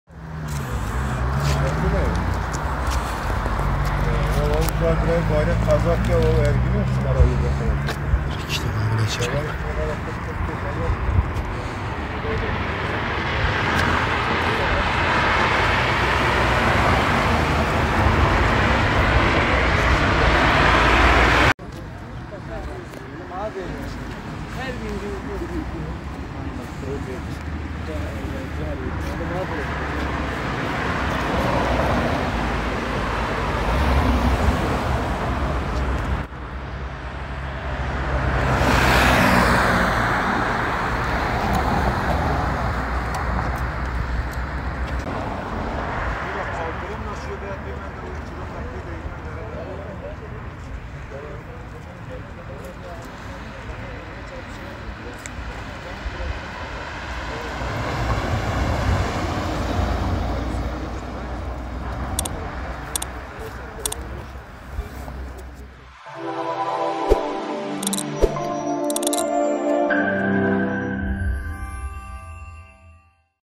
Otomobil takla attı: 3 yaralı
Bitlis merkez Batarya Kavşağı'nda, karşıdan gelen arabaya çarpmamaya çalışan otomobil takla attı. Araçta bulunan 3 kişi yaralandı.

Değerli İzleyicilerimiz;